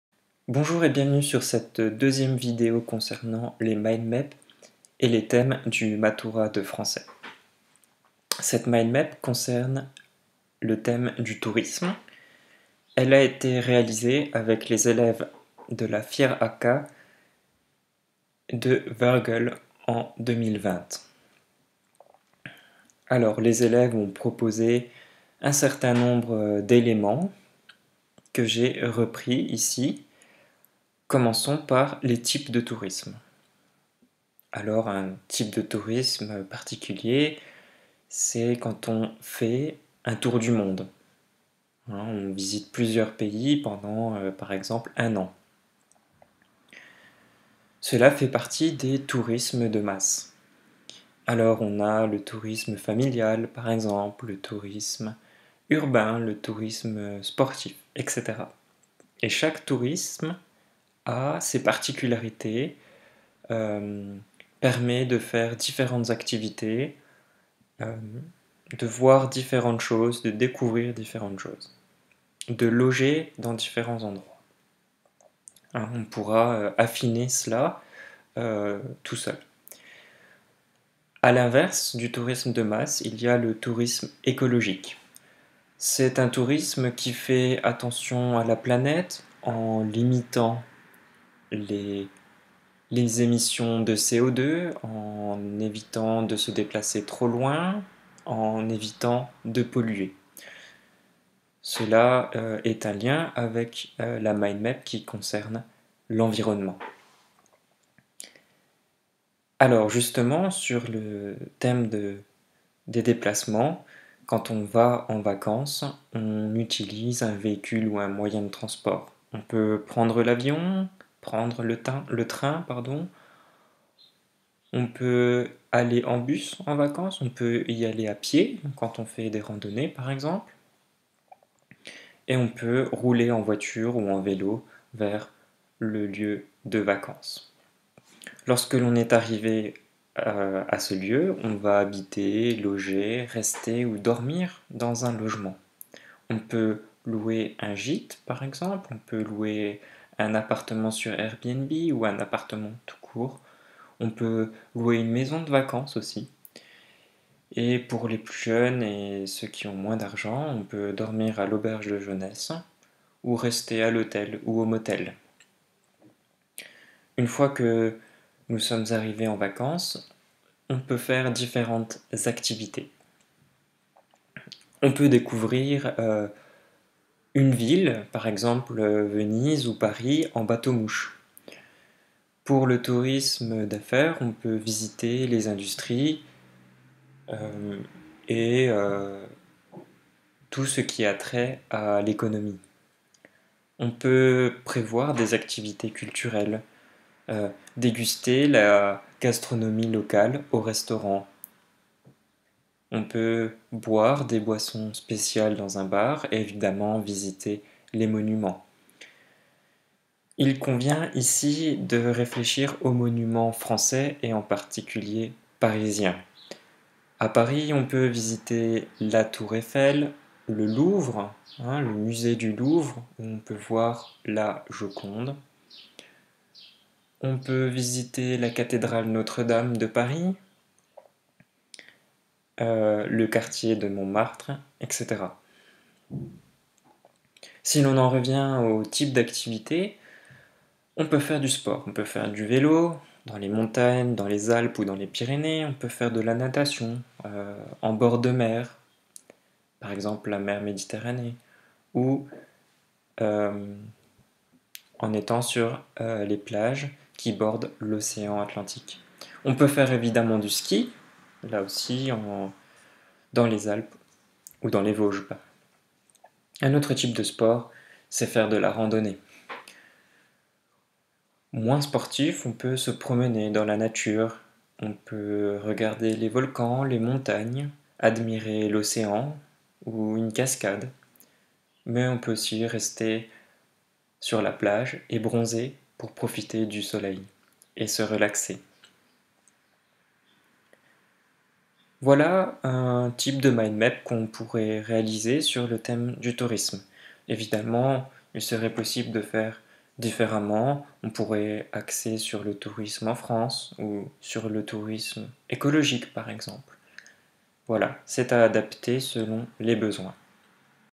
MATURA - FRZ. Thema: TOURISME (Mindmap)
Das Mindmap zum Thema "Tourisme" wird hier dargestellt und erklärt.